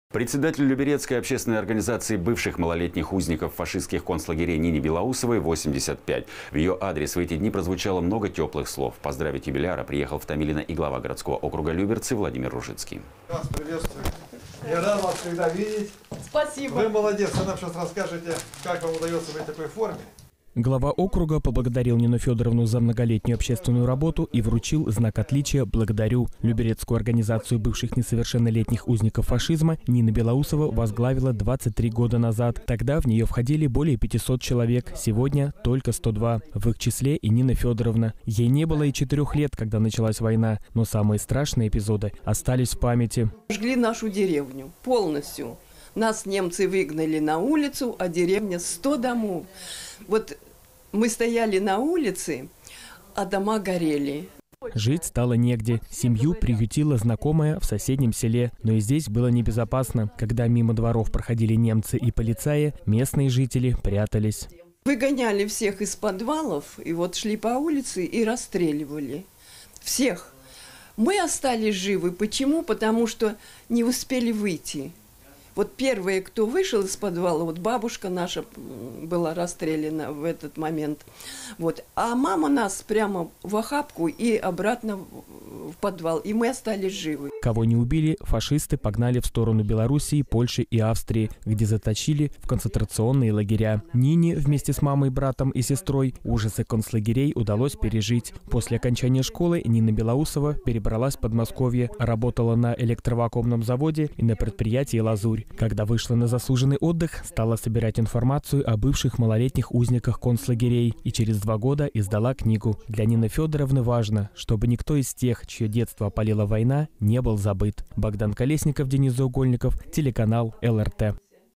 Юбилей Нины Белоусовой
Председателю Люберецкой общественной организации бывших малолетних узников фашистских концлагерей Нине Белоусовой – 85 лет. В её адрес в эти дни прозвучало много тёплых слов. Поздравить юбиляра приехал в Томилино и глава городского округа Люберцы Владимир Ружицкий.

«Я Вас приветствую, я рад Вас всегда видеть. – Спасибо. – Мы молодец. Вы нам сейчас расскажете, как Вам удаётся быть в такой форме».

Глава округа поблагодарил Нину Фёдоровну за многолетнюю общественную работу и вручил знак отличия «Благодарю». Люберецкую организацию бывших несовершеннолетних узников фашизма Нина Белоусова возглавила 23 года назад. Тогда в неё входили более 500 человек, сегодня – только 102. В их числе и Нина Фёдоровна. Ей не было и четырёх лет, когда началась война, но самые страшные эпизоды остались в памяти.

Нина Белоусова, председатель Люберецкой общественной организации бывших несовершеннолетних узников фашистских концлагерей:

«Сожгли нашу деревню. Полностью. Нас немцы выгнали на улицу, а деревню – 100 домов… Мы стояли на улице, а дома горели».

Жить стало негде. Семью приютила знакомая в соседнем селе. Но и здесь было небезопасно. Когда мимо дворов проходили немцы и полицаи, местные жители прятались.

«Выгоняли всех из подвалов, шли по улице и расстреливали. Всех! Мы остались живы потому, что не успели выйти. А первые, кто вышел из подвалов, бабушка наша была расстреляна в этот момент. А мама нас в охапку и обратно в подвал. И мы остались живы».

Кого не убили – фашисты погнали в сторону Белоруссии, Польши и Австрии, где заточили в концентрационные лагеря. Нине вместе с мамой, братом и сестрой ужасы концлагерей удалось пережить. После окончания школы Нина Белоусова перебралась в Подмосковье. Работала на электровакуумном заводе и на предприятии «Лазурь». Когда вышла на заслуженный отдых – стала собирать информацию о бывших малолетних узниках концлагерей. И через два года издала книгу. Для Нины Фёдоровны важно, чтобы никто из тех, чьё детство опалила война, не был забыт.